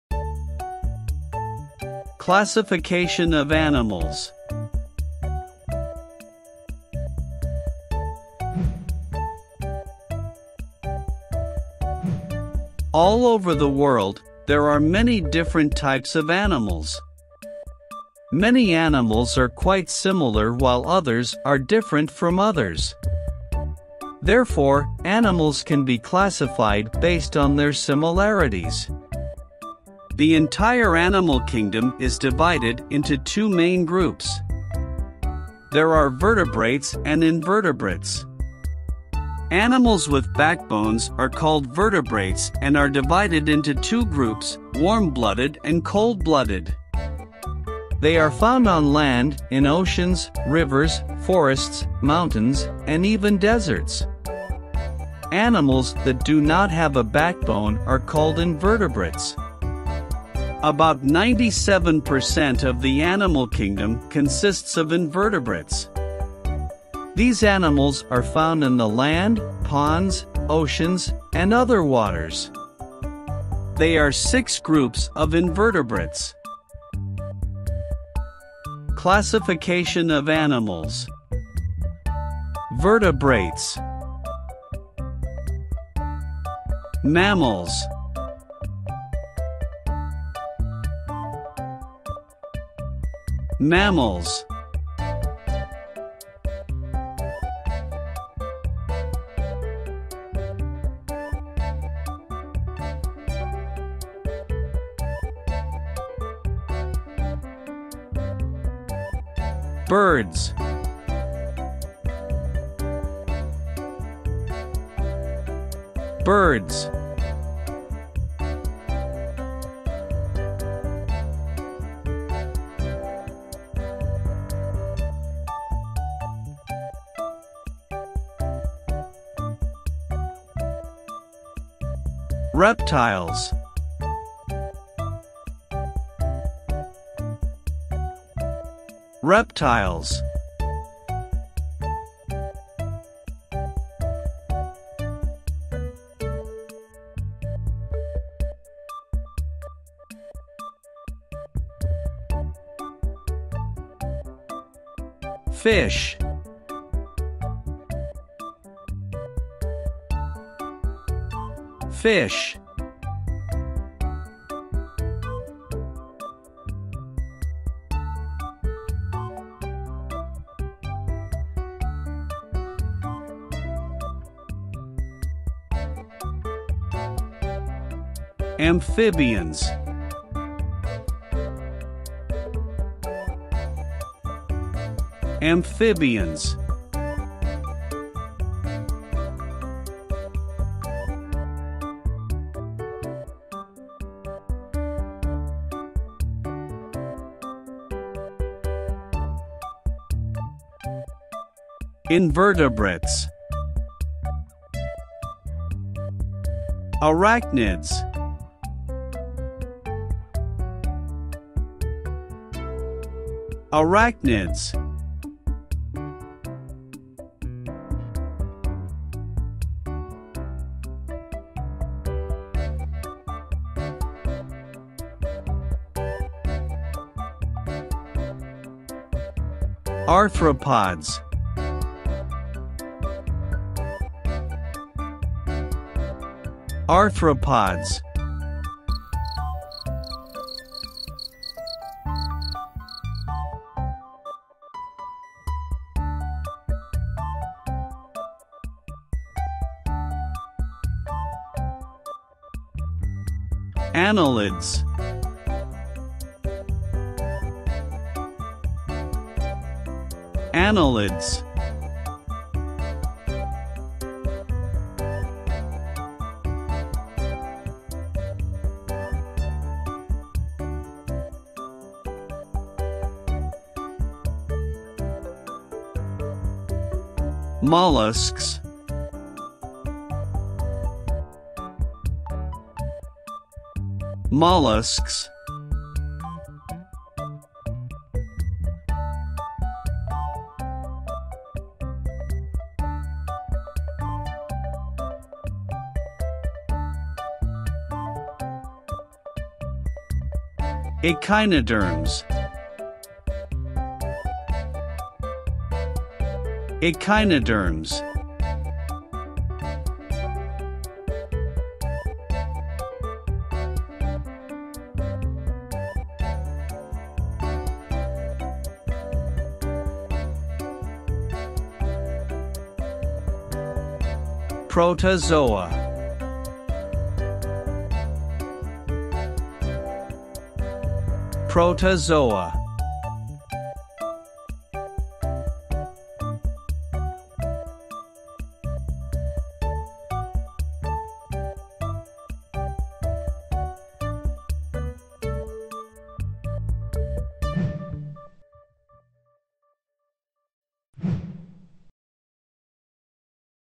Classification of Animals, learn animals classification, Classification of animals of the world
In this video, learn how to classify animals based on their physical characteristics. This step-by-step guide uses a simple and understandable approach that even children can follow. By the end of this video, you will be able to classify animals into different groups such as mammals, reptiles, and birds.